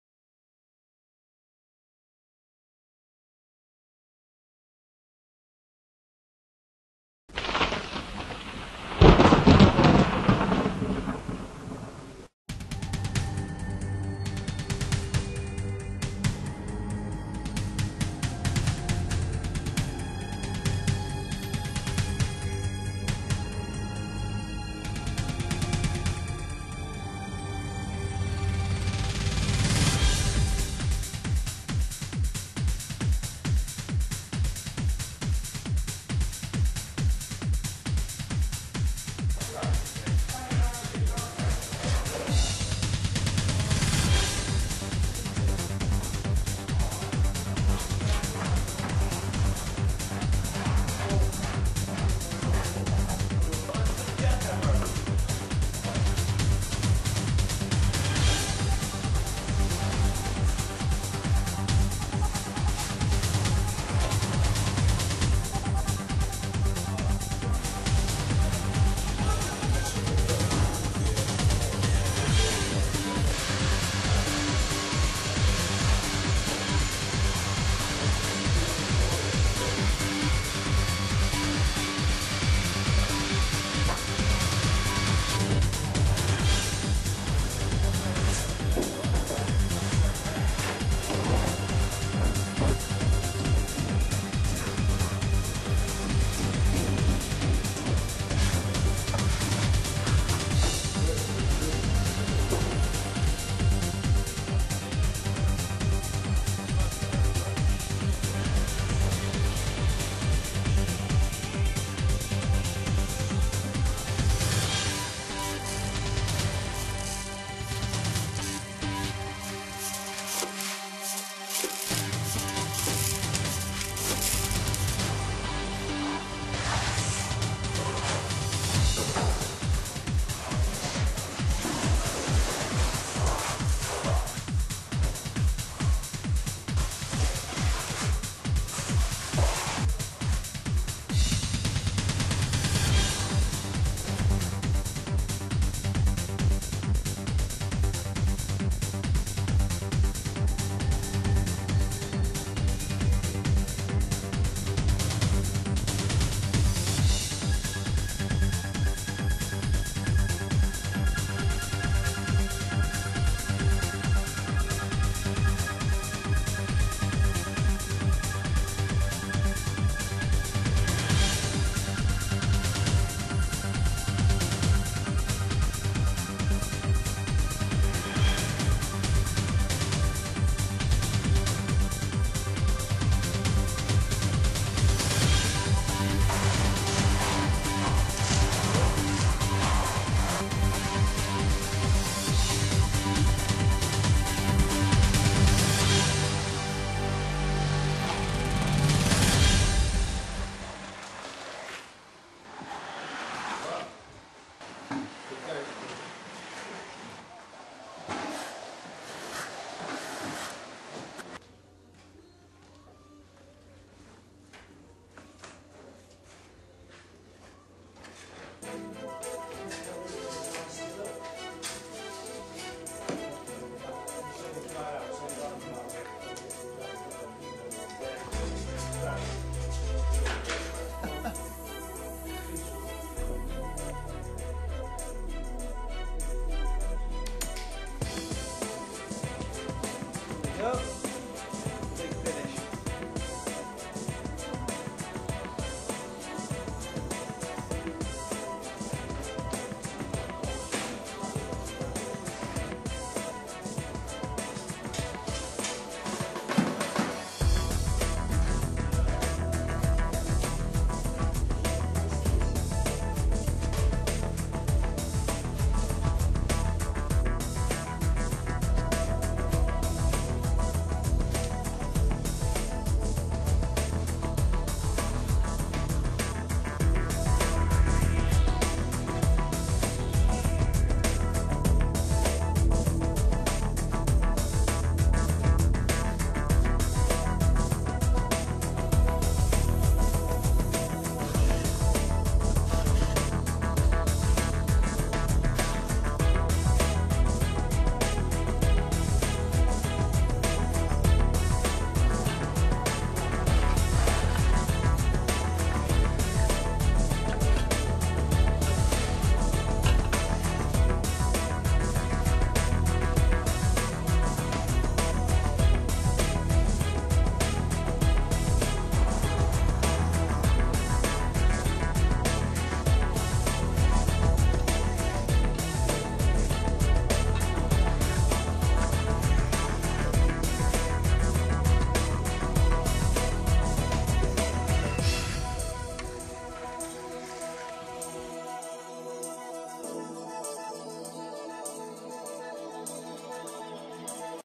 Goldtrowel trowel ocupations
short clip of footage taken at the goldtrowel training centre with students solid plastering, fibrous plastering, venetian plastering, marmorino pargetting, dashing and tiling